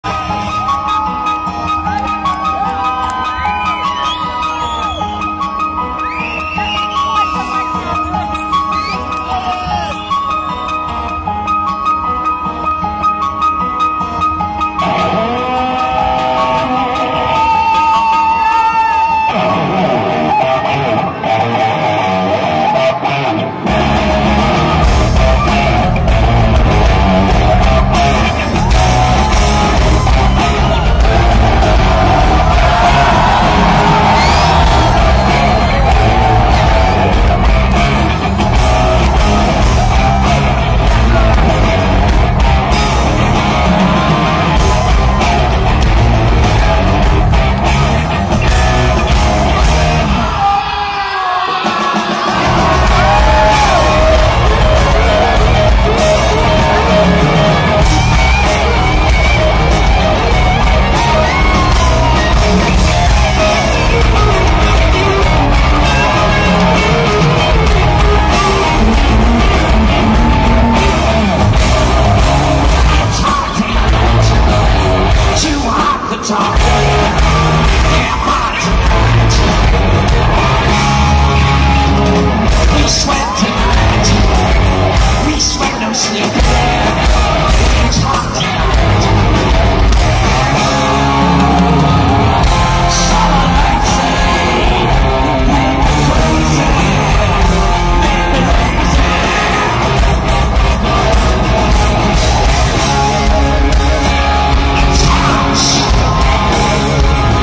Alice Cooper enters the stage, Berlin 08
Alice Cooper's entrance at the Max Schmeling Halle, Berlin. 01.12.08